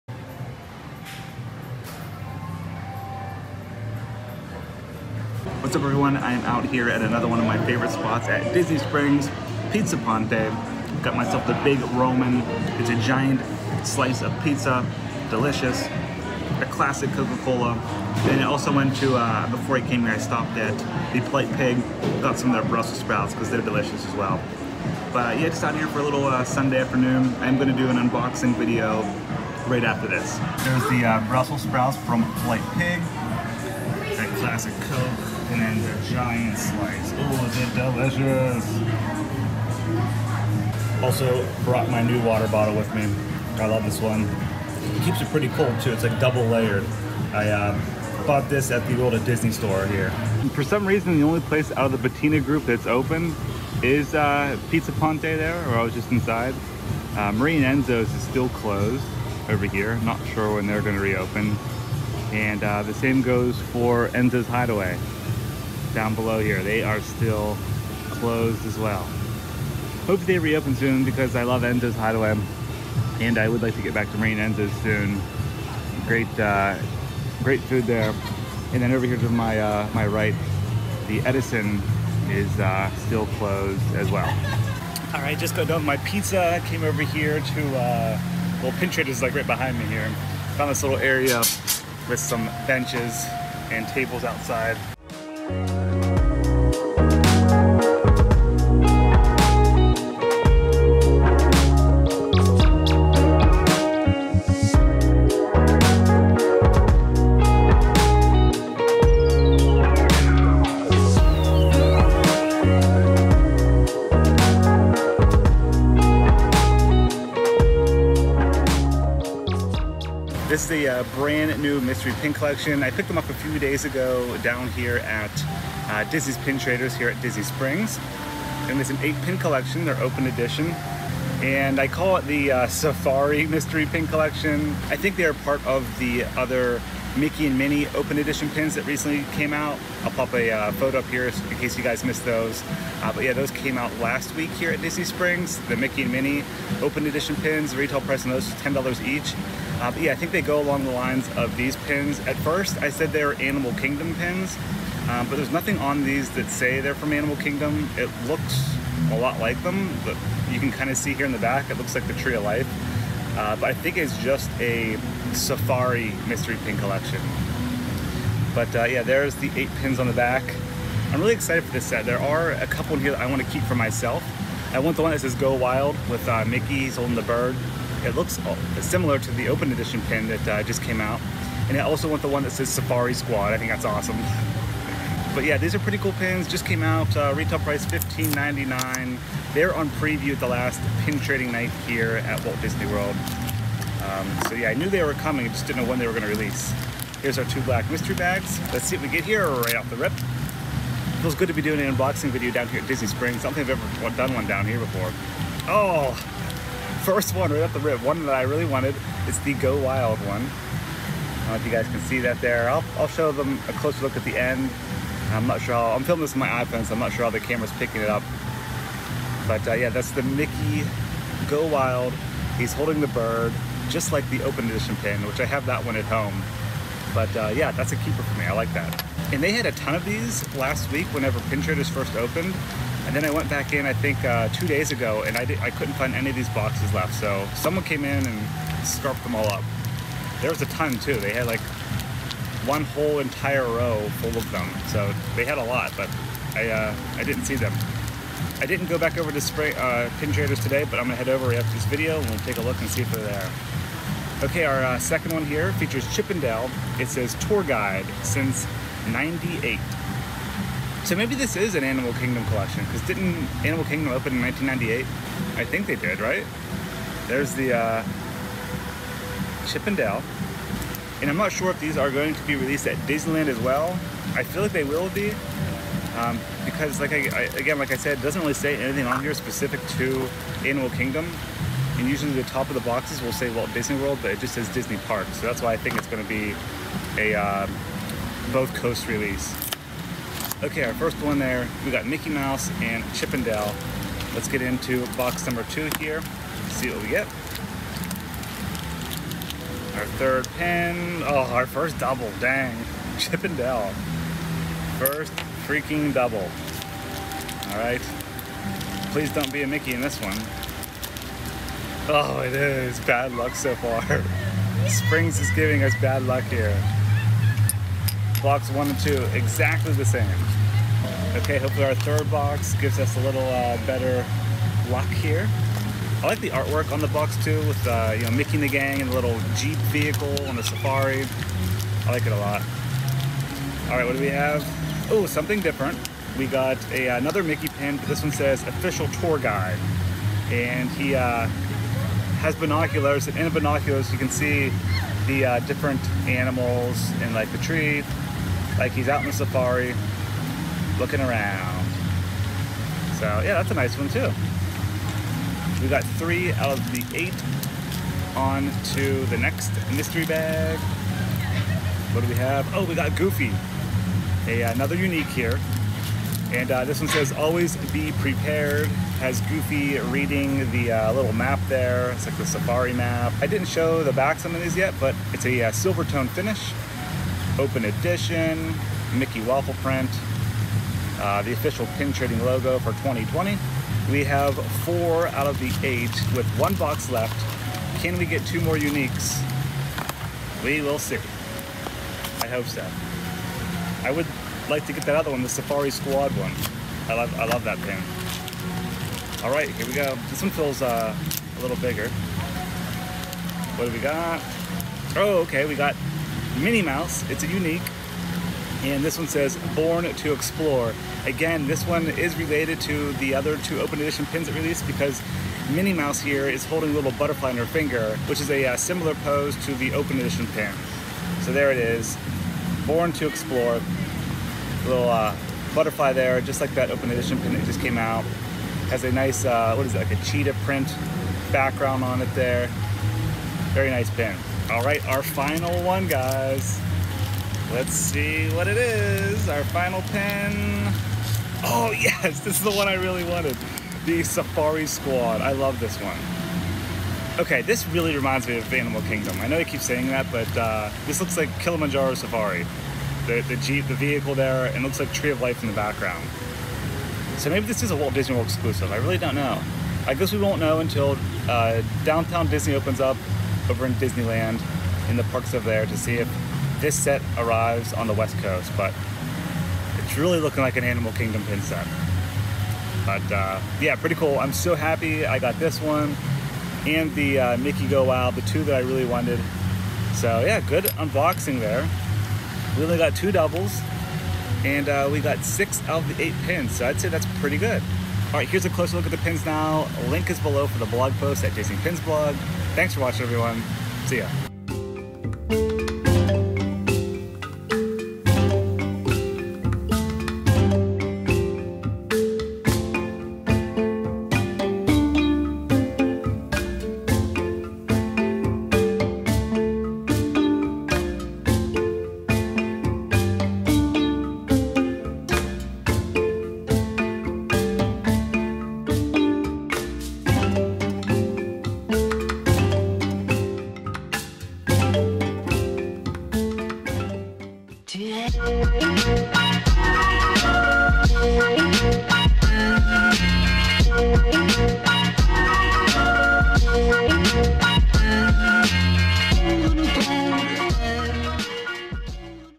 New Safari Disney Mystery Pin Unboxing!
Here is a look at the Safari Disney mystery pin set from Disney Parks! Features 8 pins in total. Released in June 2020 at Disney Springs.